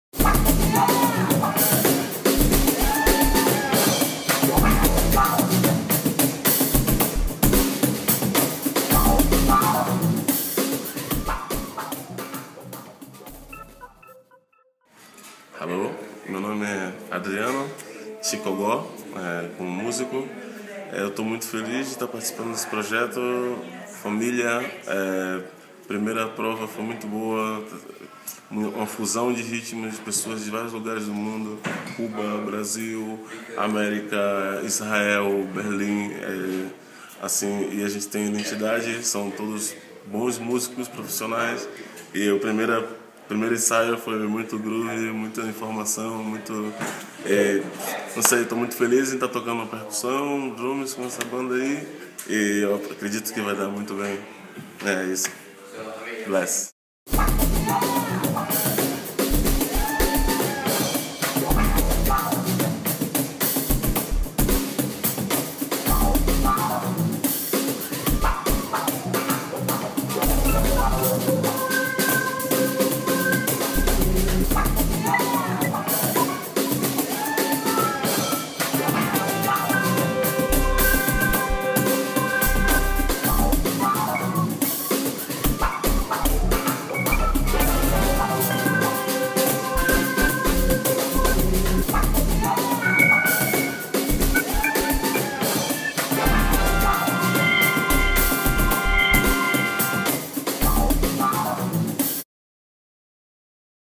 Introducing "The Family": "MC TIKOGÒ Shoutout"
MC TIKOGÒ (aka Adriano Gótico) is without question one of the most gifted and talented artists working in Berlin today. He is originally from the Itapuã neighborhood in Salvador de Bahia. He started playing music as a child with "broomsticks" and home-made drums. He became a professional musician at age 17 after performing on stage with the great Nana Vasconcelos. MC TIKOGÒ has studied with masters such as Lula Nascimento and Eddu Nascimento et Anuniciação. He is constantly experimenting, producing and pushing the boundaries of one of his favorite instruments: the Pandeiro. MC TIKOGÒ is one of the many magnificent musical minds of the new Berlin supergroup "The Family".